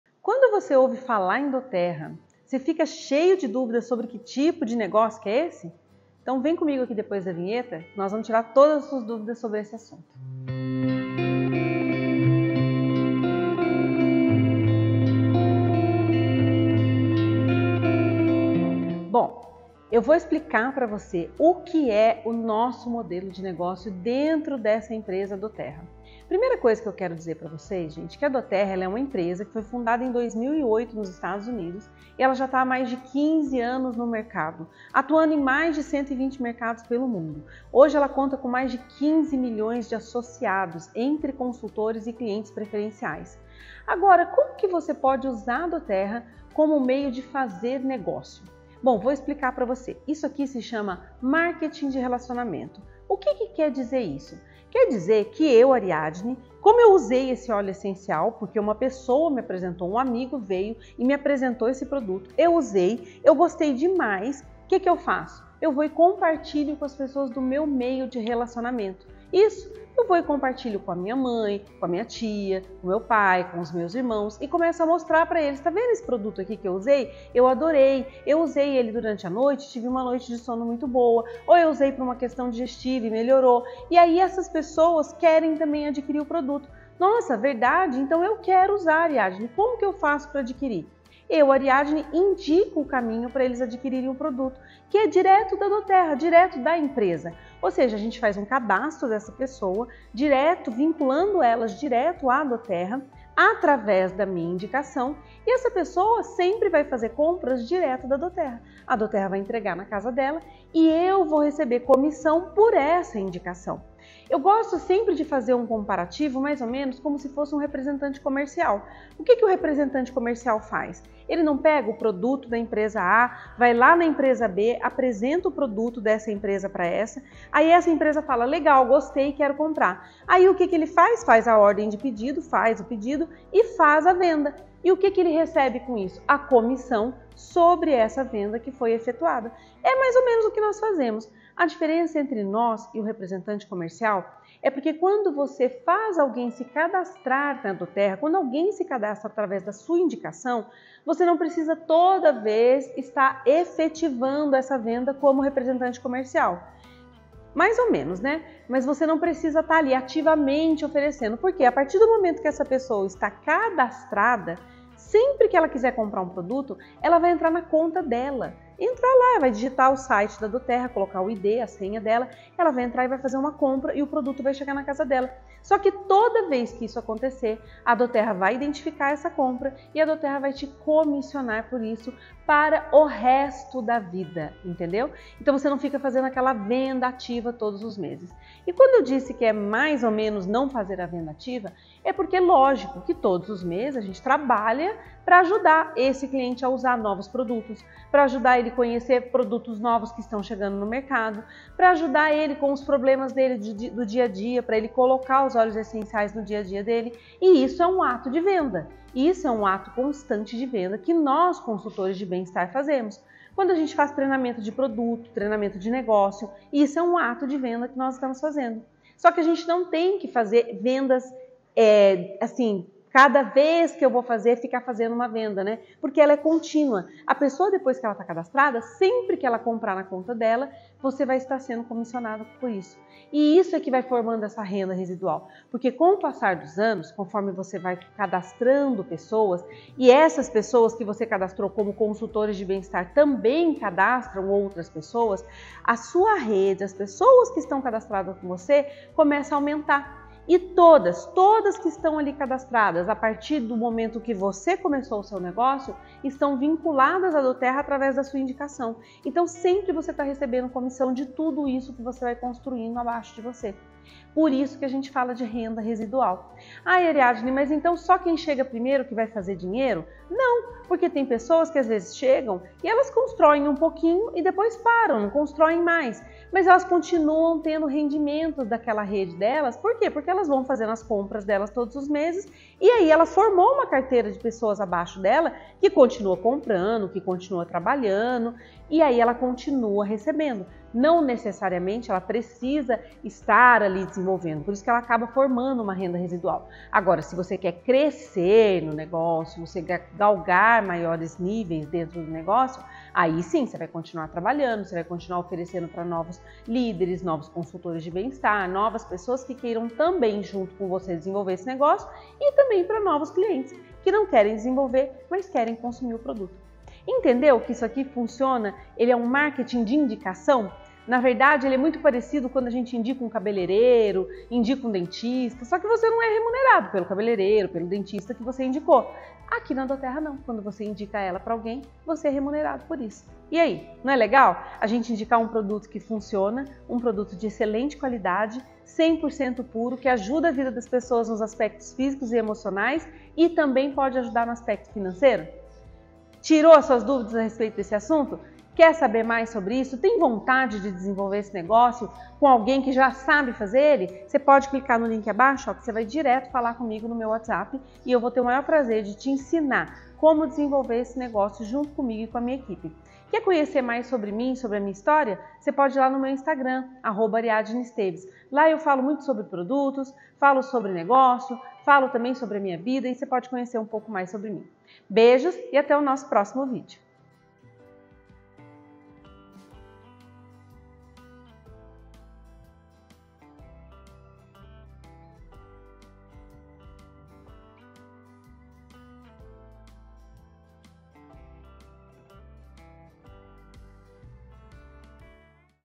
Empreenda com dōTerra: Tudo que você precisa saber - com Ariadne Esteves
* Quer SABER MAIS sobre os óleos, ou sobre como começar seu negócio?

* Não quer perder nenhum vídeo?